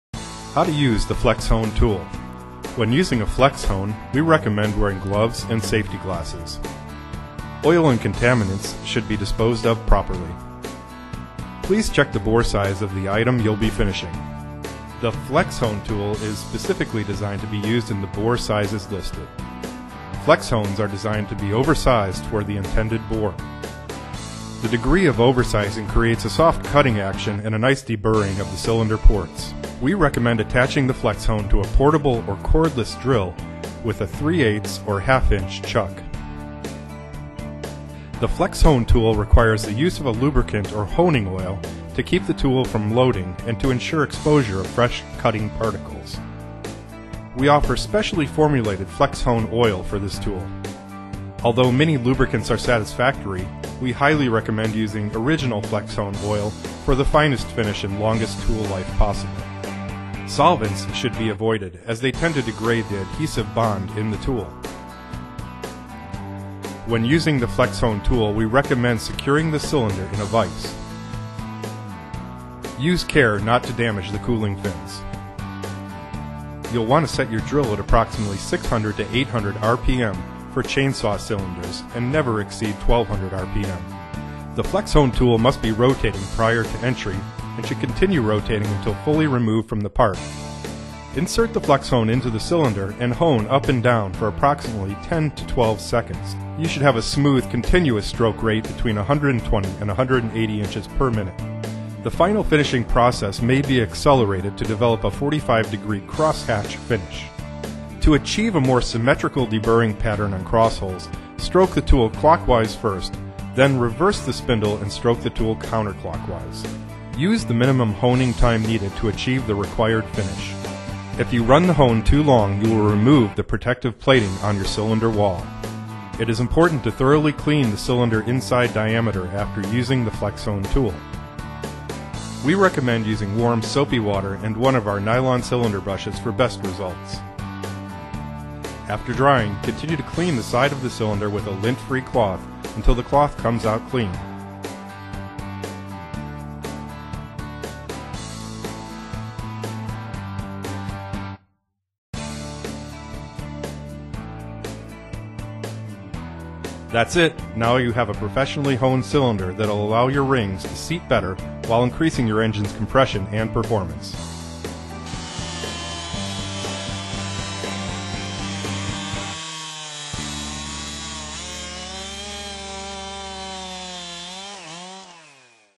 Flex Hone Demonstration Video
-- FREE CATALOG of Firewood & Woodcutting Tools, Chainsaws, Tree Climbing Gear, Arborist Supplies and Outdoor Power Equipment -- If you take your engine repairs seriously, you're going to want a set of these. Flex-hones are used to lightly surface the bore of your cylinder, creating a cross hatch of scratches that helps your rings seat tightly to increase compression. They also smooth over imperfections around intake and exhaust ports. This type is 240 grit aluminum oxide (AO), recommended for cylinder coatings such as chrome and Nikasil. We also offer the more industrial 320 grit silicon carbide (SC) which works on all surfaces including cast iron cylinders. Honing is done by attaching a well oiled flex hone to a hand held drill (at 600-800 RPM) and oscillating the hone up and down 8 to 10 strokes for approximately 15-20 seconds. Hones are slightly larger than bore size.